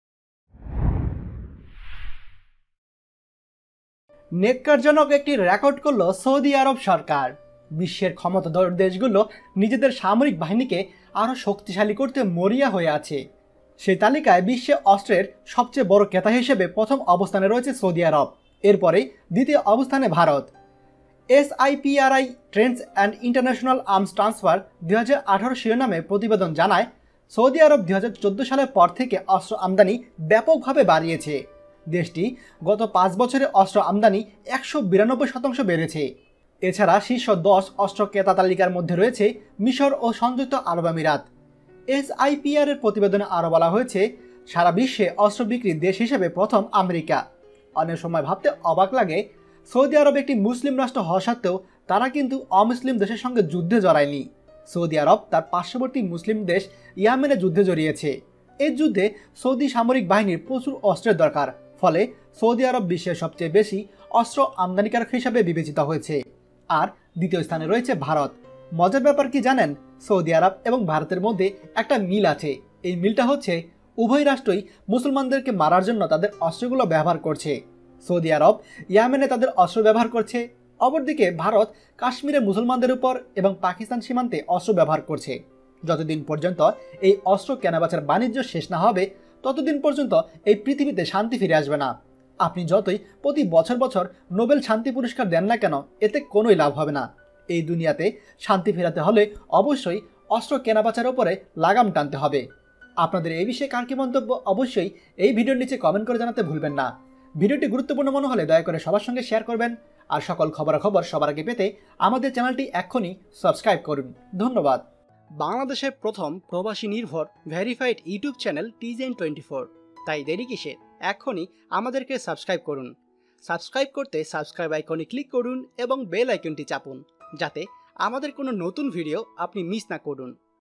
ঘৃণ্যতম রেকর্ড করলো সৌদি আরব সরকার। বিশ্বব্যাপী অস্ত্রের ক্রেতা হিসেবে প্রথম অবস্থানে সৌদি আরব।
বিশ্বের ক্ষমতাধর দেশগুলো নিজেদের সামরিক বাহিনীকে আরও শক্তিশালী করতে মরিয়া হয়ে আছে। সেই তালিকায় বিশ্বে অস্ত্রের সবচেয়ে বড় ক্রেতা হিসেবে প্রথম অবস্থানে রয়েছে সৌদি আরব। এরপরই দ্বিতীয় অবস্থানে ভারত।